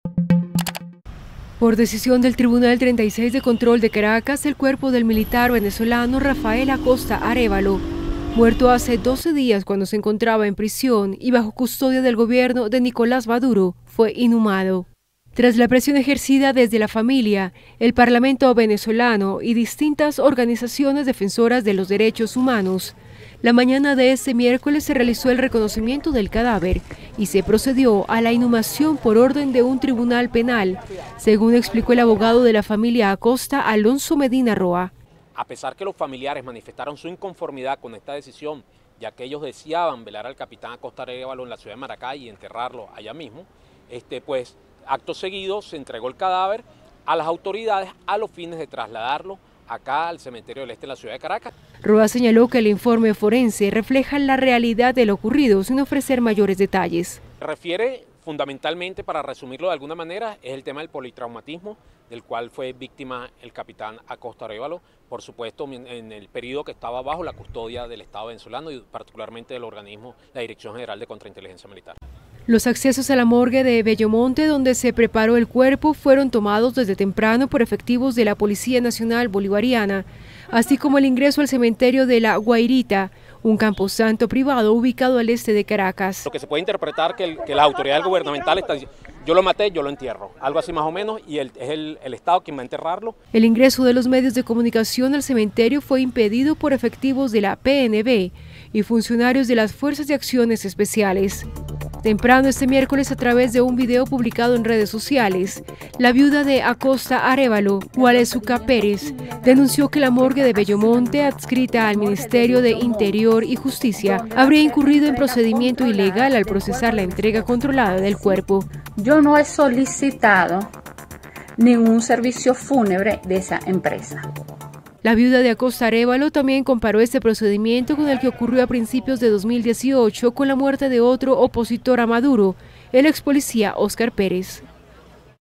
Tribunal penal de Caracas ordena inhumar al militar muerto en prisión
Caracas, 10 jul (EFE).- El cuerpo del militar venezolano Rafael Acosta Arévalo, muerto hace 12 días cuando se encontraba en prisión y bajo custodia del Gobierno de Nicolás Maduro, fue inhumado este miércoles por orden de un tribunal de Caracas, informó el abogado de la familia Acosta, Alonso Medina Roa.
Imágenes: Andry Rincón
Edición y locución: Annia Hidalgo Vargas